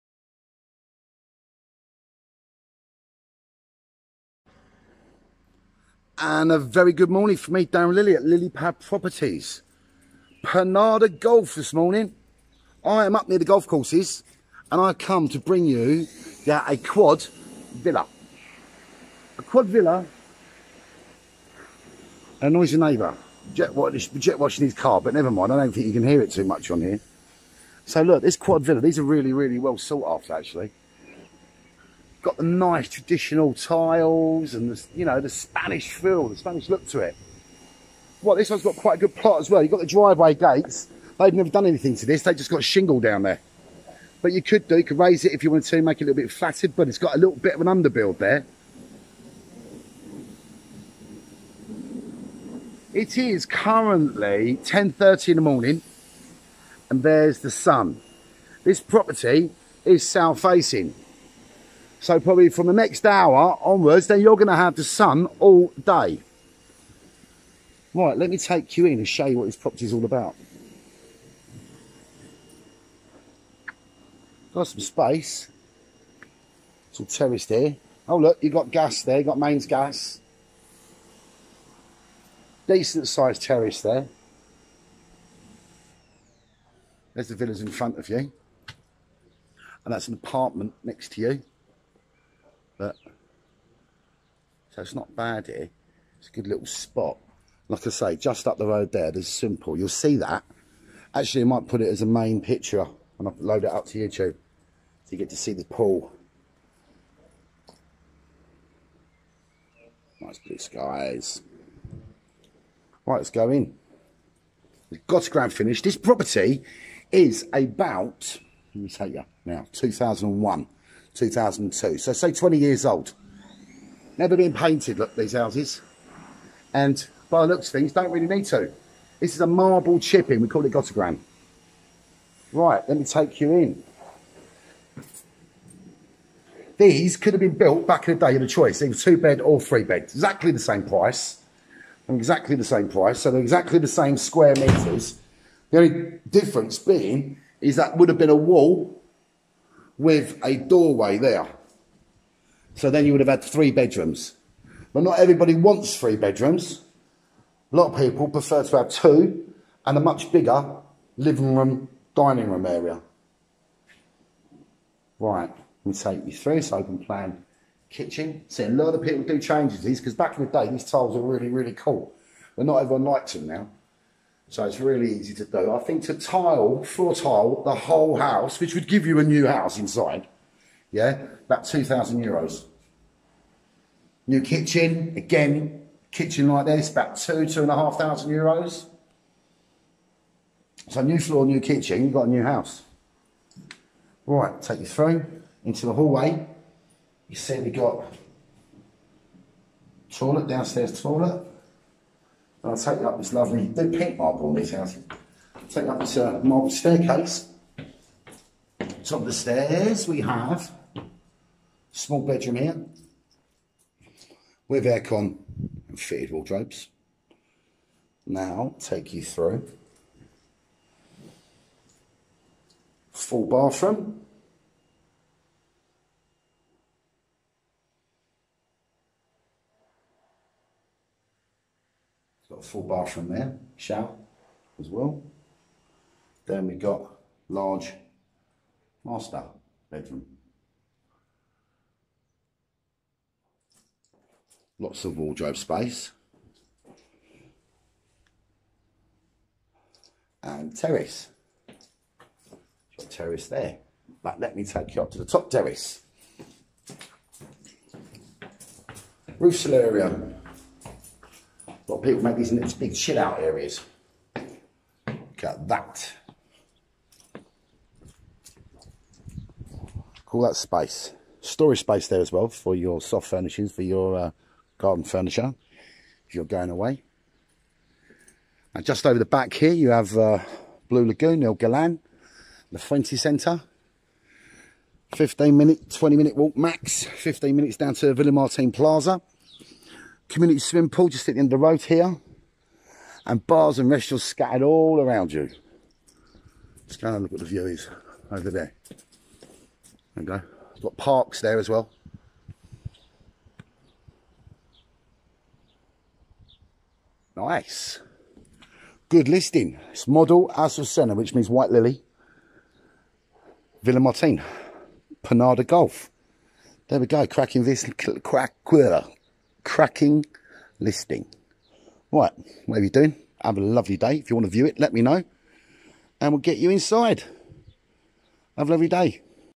SOLD Just 129,950€ 2 bed Quad Villa in Pinada Golf Villamartin, South facing
LilyPad Properties Spain are pleased to offer you this property on the popular Orihuela Costa on the Costa Blanca, For more information you can email me on or message me on watts app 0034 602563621